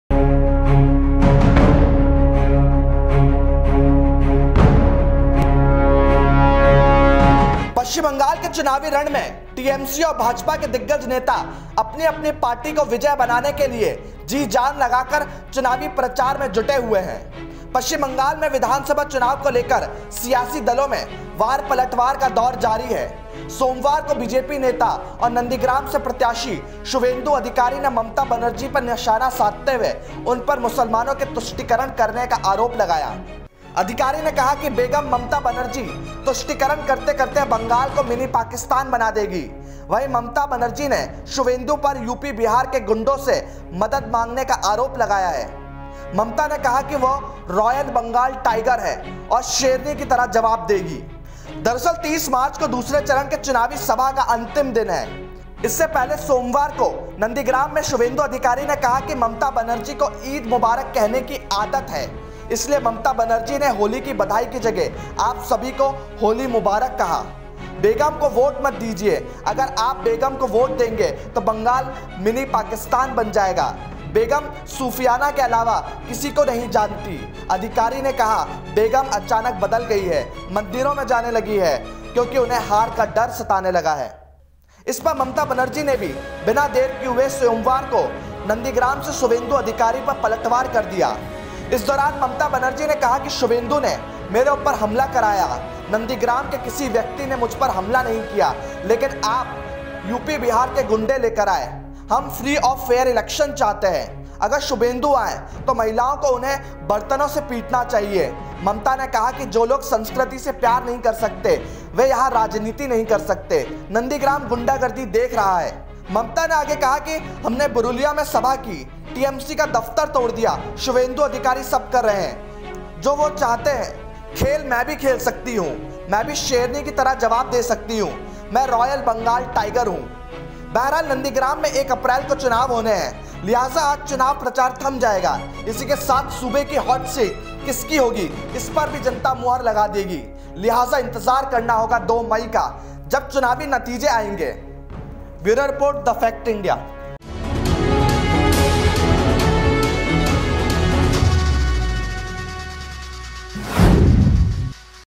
West Bengal : Suvendu Adhikari ने दीदी को बताया ‘बेगम’ तो Mamata Banerjee ने किया जबरदस्त पलटवार
West Bengal Assembly Elections 2021 में TMC और BJP के दिग्गज नेता अपनी पार्टी को विजयी बनाने के लिए से जी-जान लगाकर चुनाव प्रचार करने में जुटे हुए हैं... West Bengal Assembly Elections 2021 को लेकर राजनीतिक दलों में वार-पलटवार का दौर जारी है... Monday को BJP नेता और Nandigram से प्रत्याशी Suvendu Adhikari ने Mamata Banerjee पर निशाना साधते हुए उन पर Muslims का तुष्टीकरण करने का आरोप लगाया...